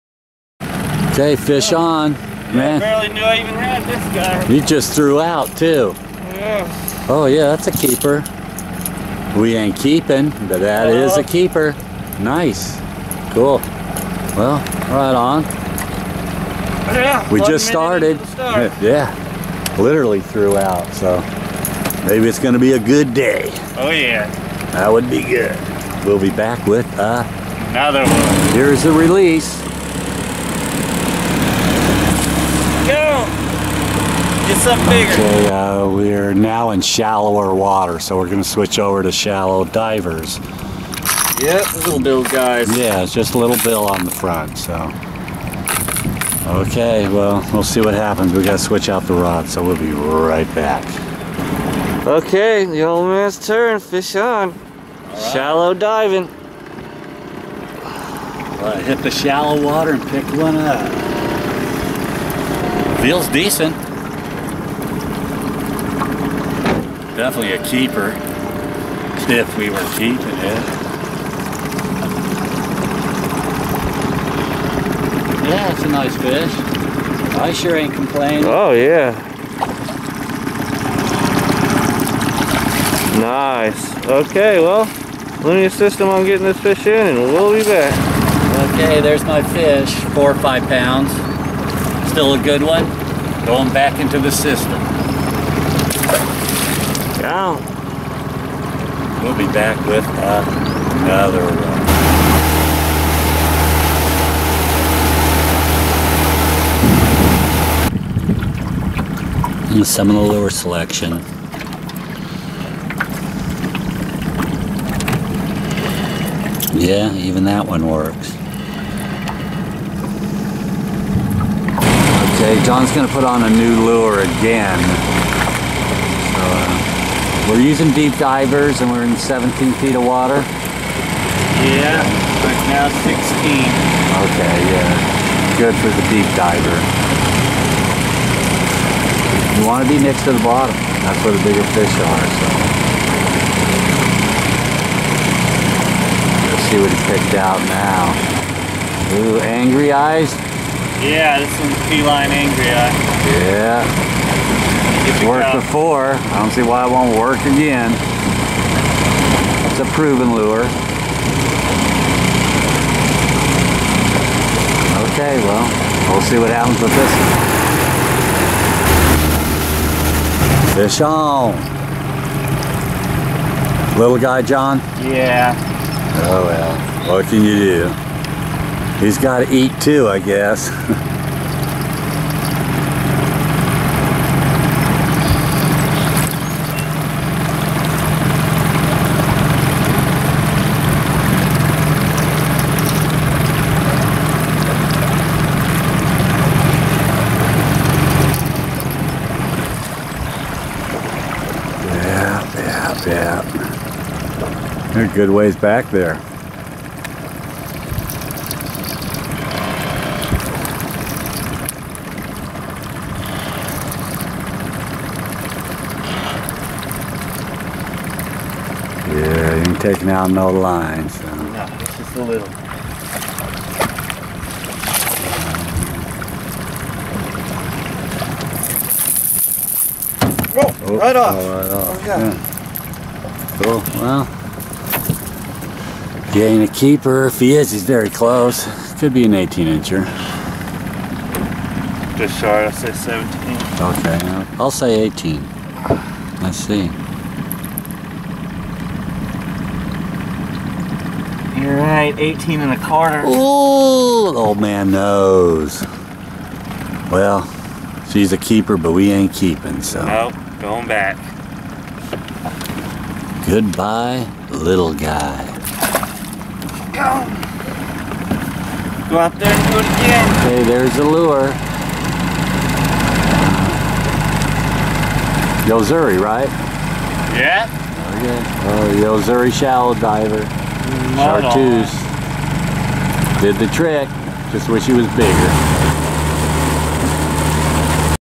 STRIPER TROLL IN RIO !
RIO VISTA.. STRIPED BASS ACTION IS STILL ON.. W/ MORE BASS STILL MIGRATING UP THE SACRAMENTO RIVER THE BITE CAN BE GOOD W/ LURES & CUT BAIT. WE TROLLED FOR AWHILE WITH NOT A LOT OF TAKERS ..MOST FISH WERE ELSE WHERE ..NOT SO MUCH IN OUR AREA WE WERE TROLLING..BUT THAT'S FISHING !   ON THE CALIF. DELTA